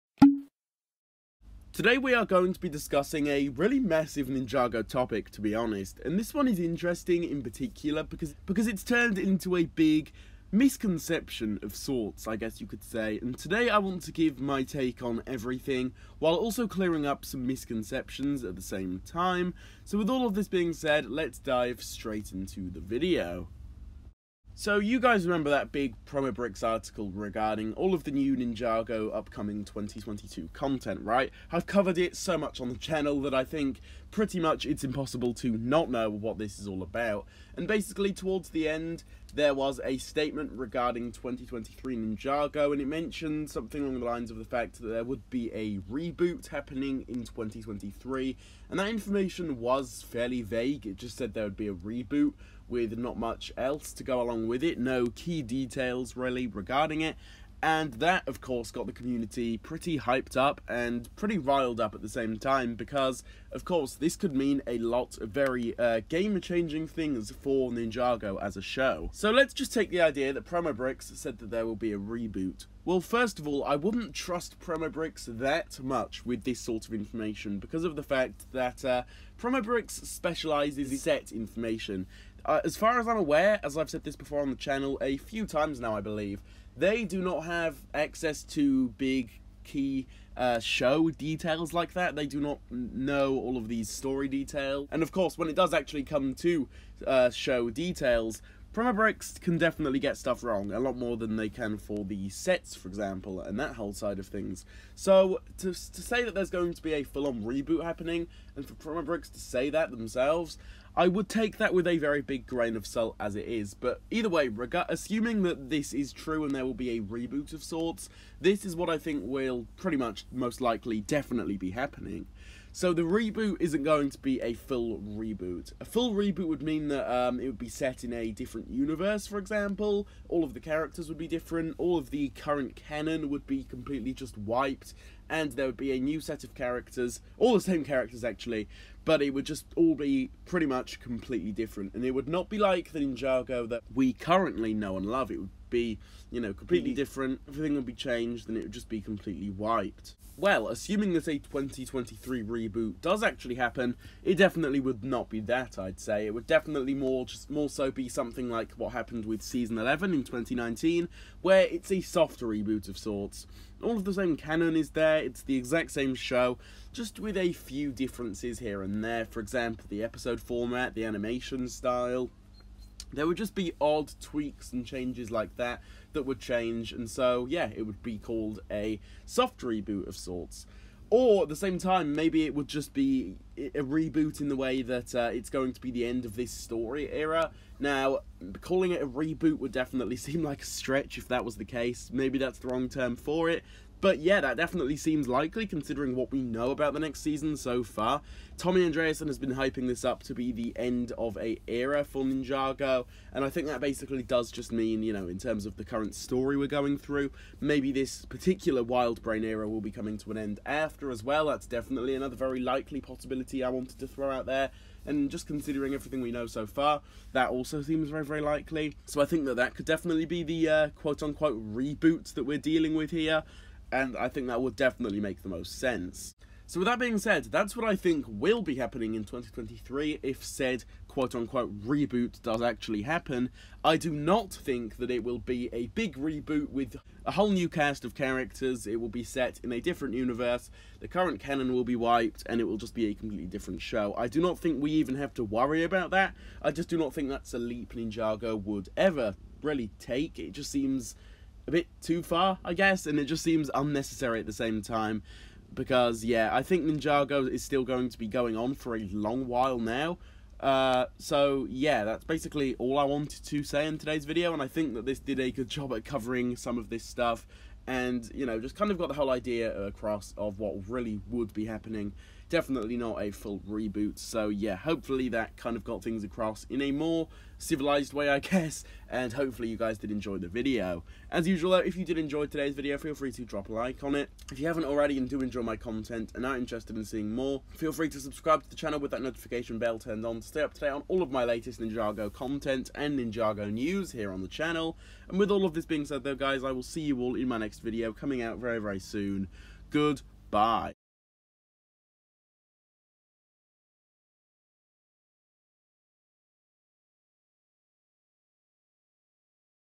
This Would Change Ninjago FOREVER! - Ninjago Reboot Details Explained & My Thoughts!
In today’s video, we discuss the Ninjago 2023 “reboot” and how it may not be as big as some people may think… I hope you all enjoy the video!

Copyright